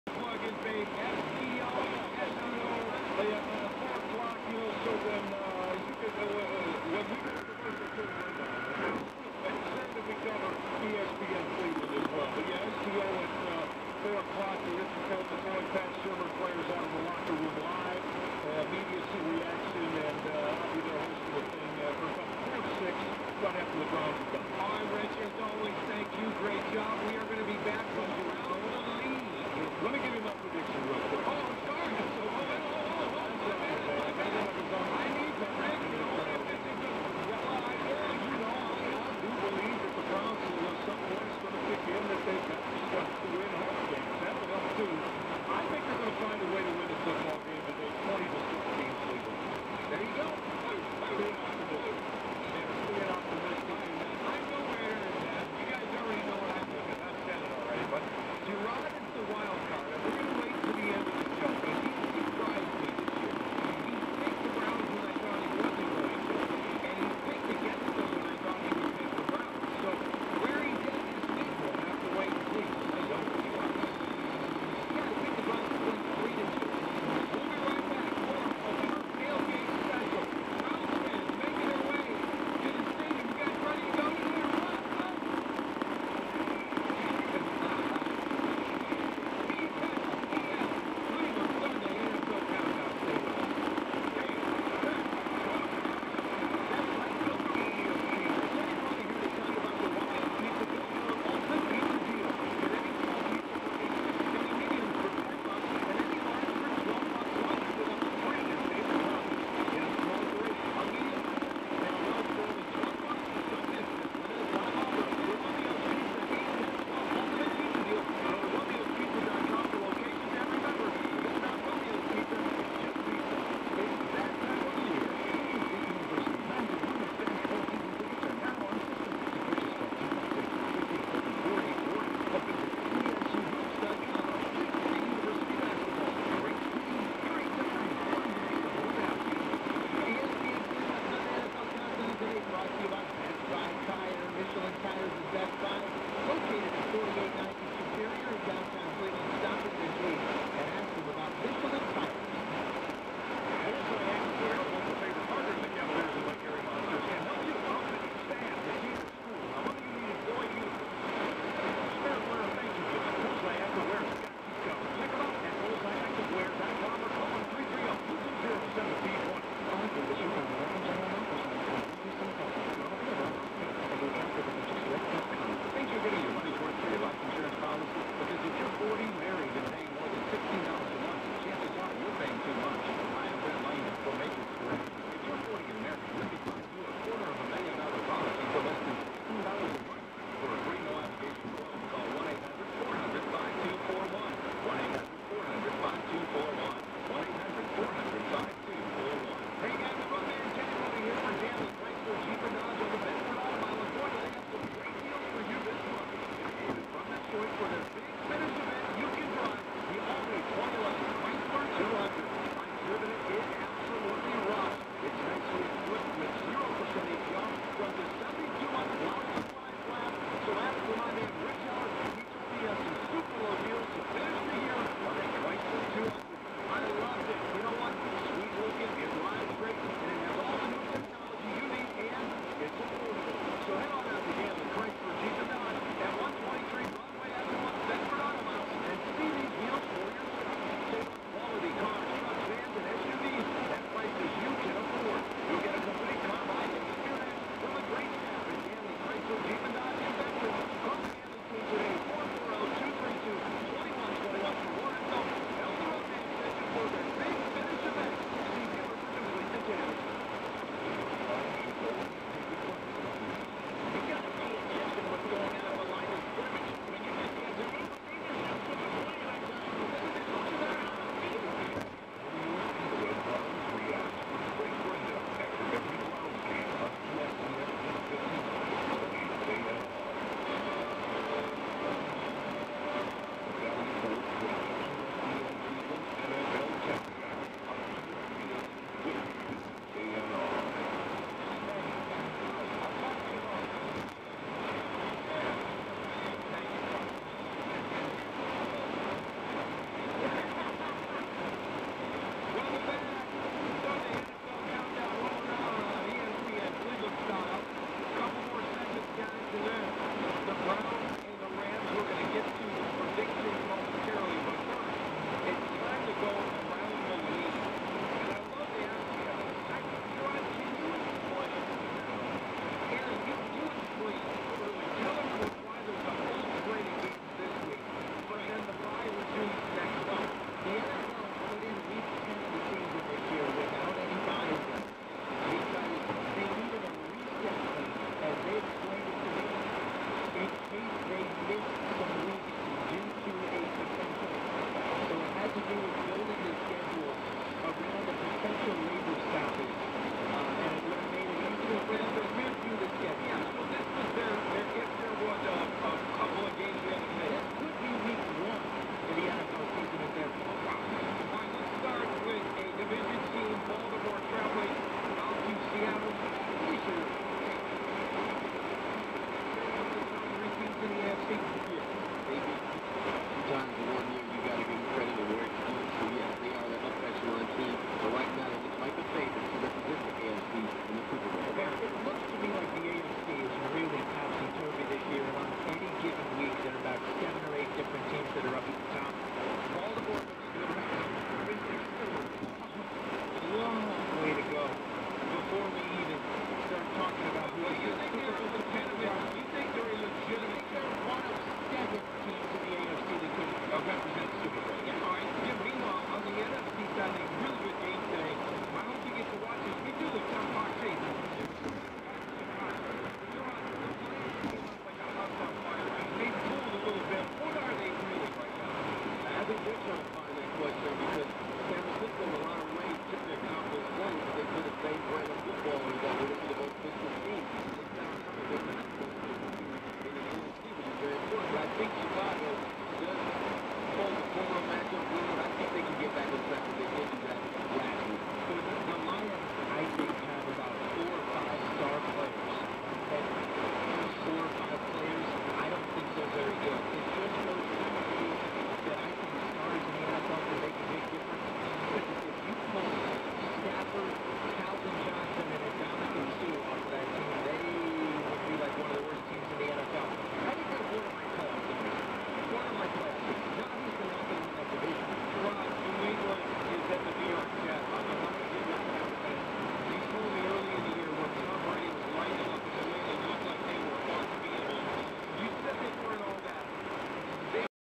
East Outerbelt Freeway (Interstate 271 Exits 34 to 26) southbound
The Eastern Outerbelt links Cleveland's eastern suburbs with the major east-west highways in the region.

Highlights: JCT I-480